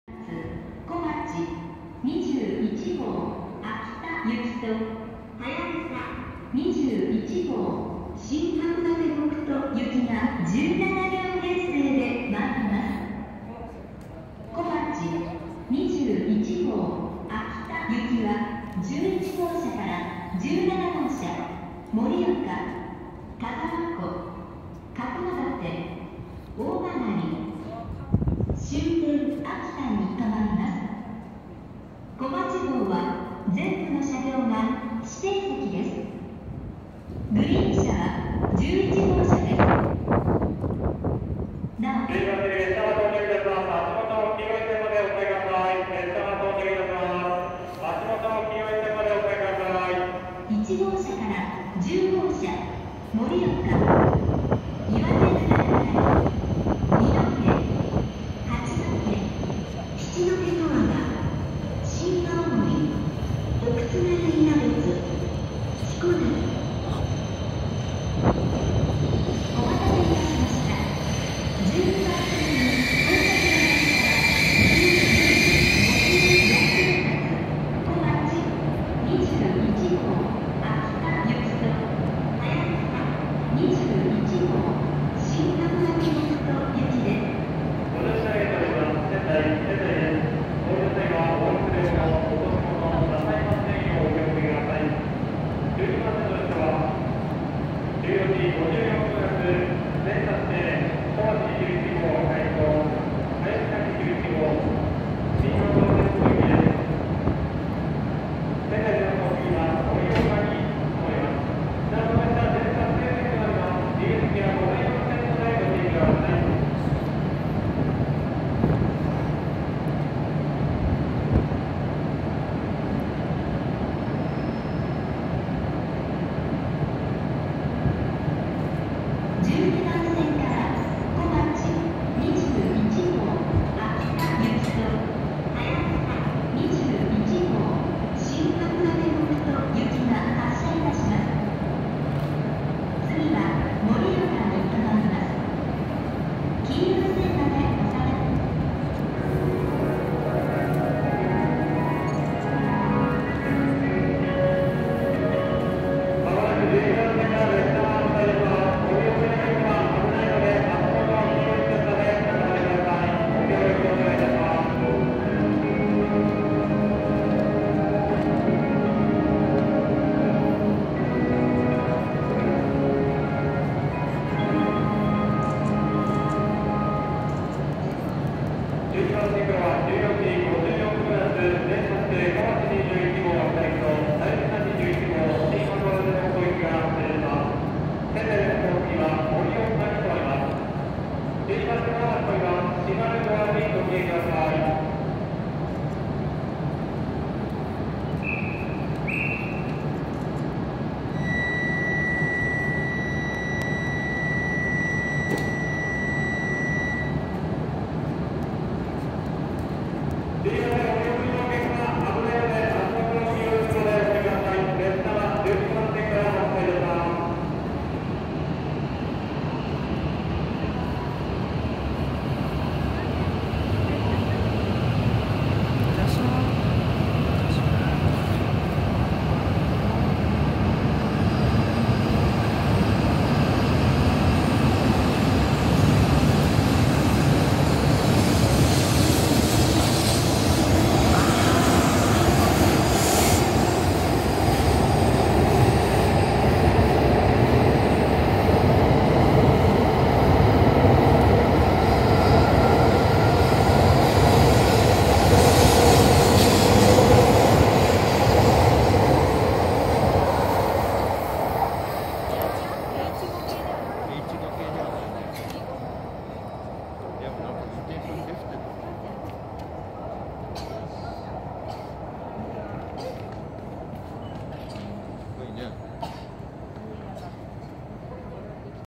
北海道新幹線 E6系+E5系 こまち号秋田行 はやぶさ号新函館北斗行 仙台駅発着
下り電車がきたのですがH5系ではありませんでした。